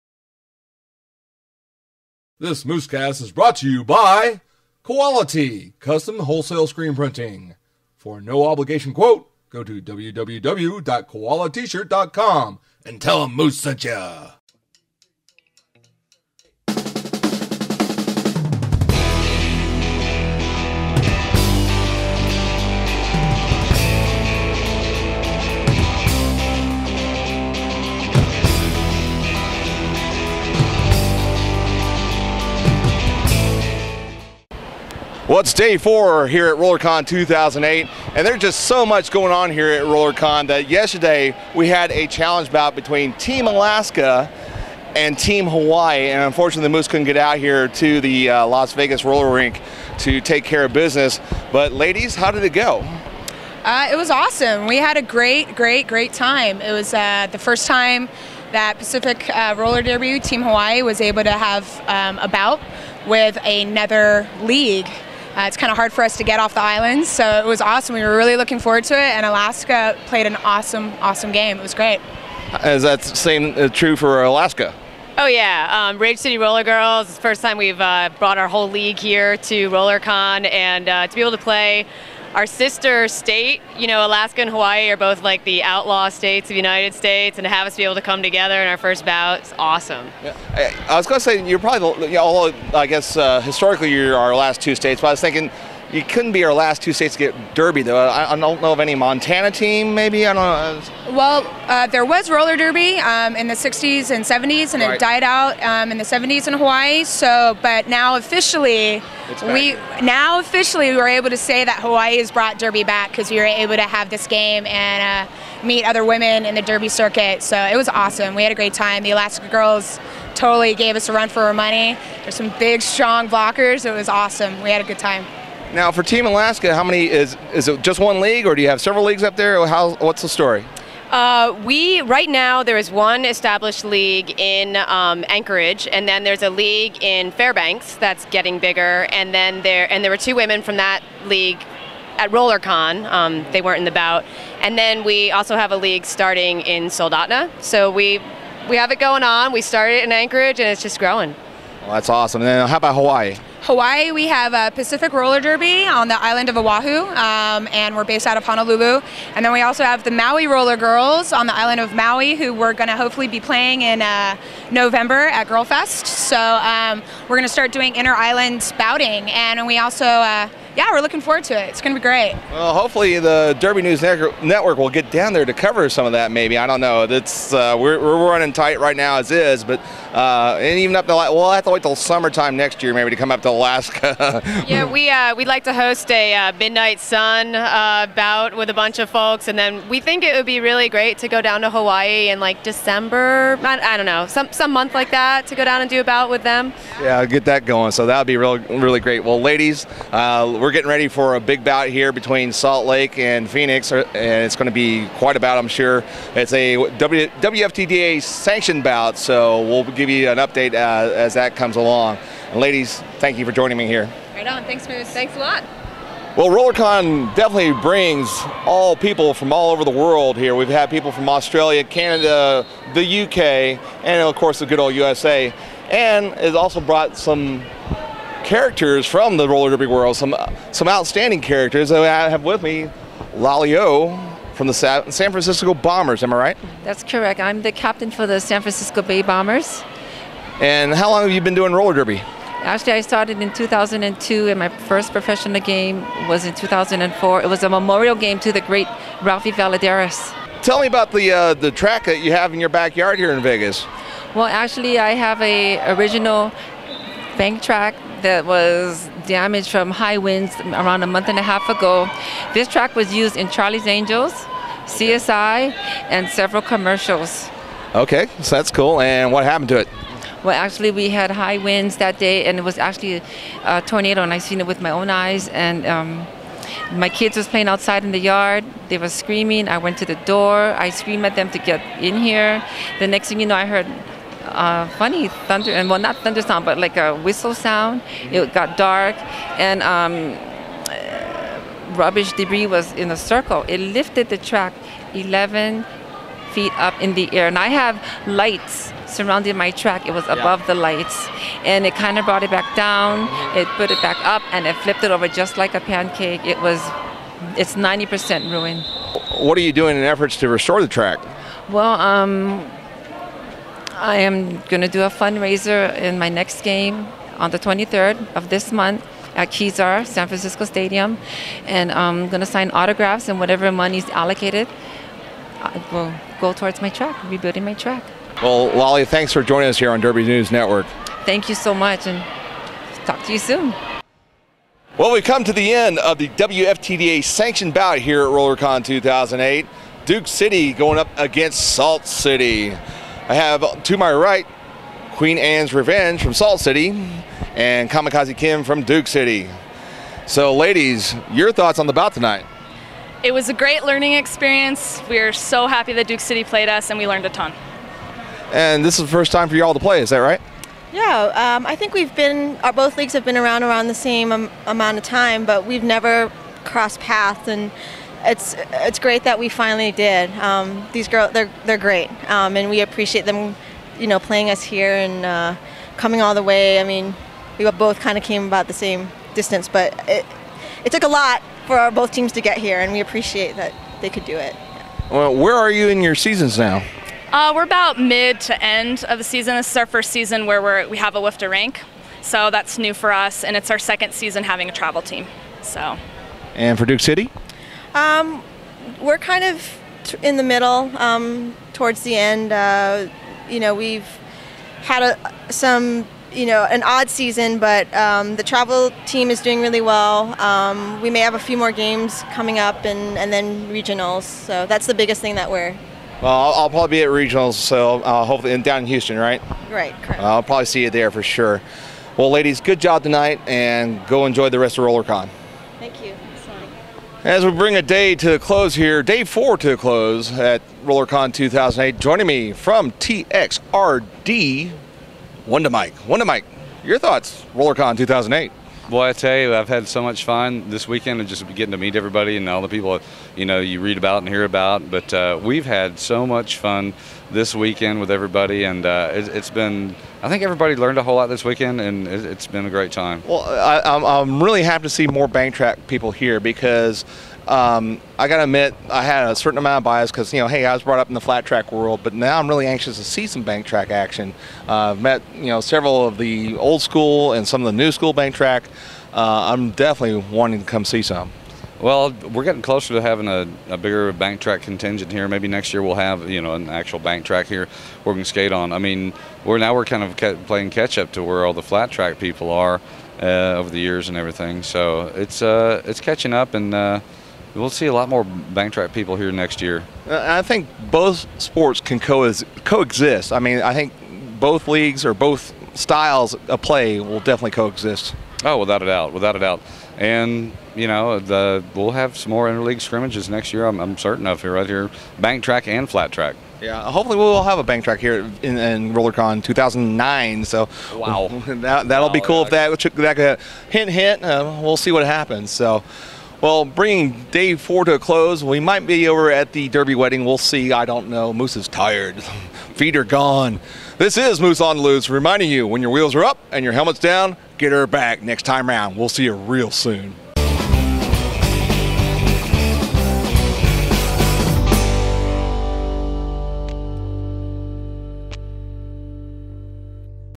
RollerCon 2008 Day 4
Moose Ondaluce interviews:
Gypsy Pickles - Team Hawaii
Carr Wreck - Team Alaska
Lali O - SF Bombers
Queen Anne's Rev. - Captain Salt City
Kamikazi Kim - Captain Duke City
WundaMike - Announcer TXRD